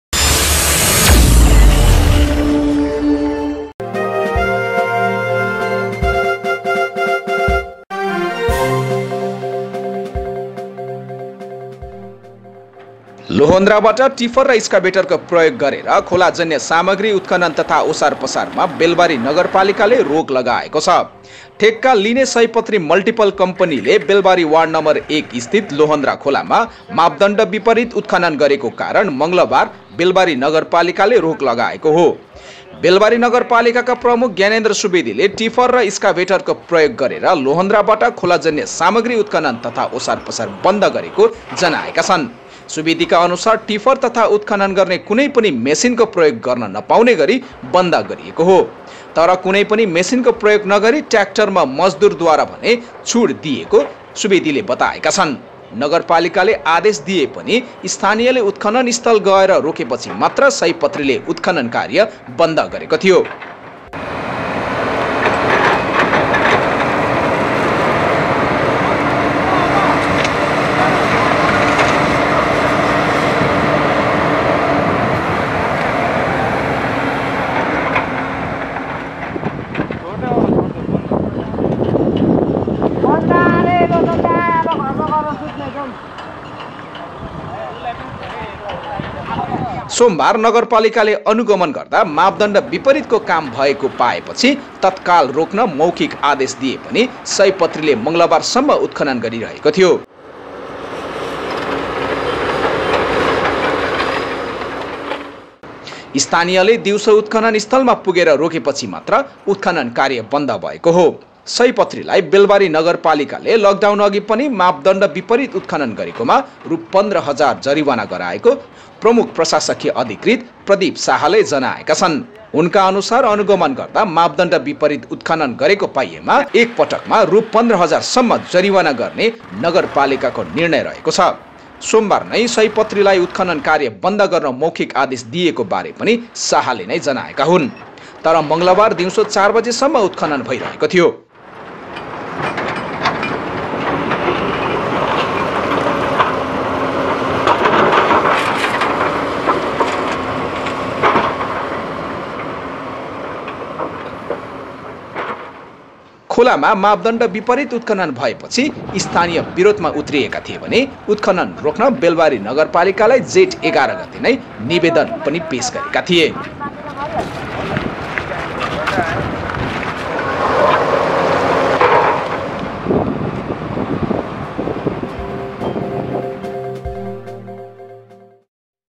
लोहोन्द्रामा टिपर र स्काभेटर प्रयोगबाट खोलाजन्य सामाग्री उत्खनन गर्न बन्दा || Belbari || Lohondra
KN_NEWS_लोहोन्द्रामा टिपर र स्काभेटर प्रयोगबाट खोलाजन्य सामाग्री उत्खनन गर्न बन्दा || Belbari || Lohondra
post production :-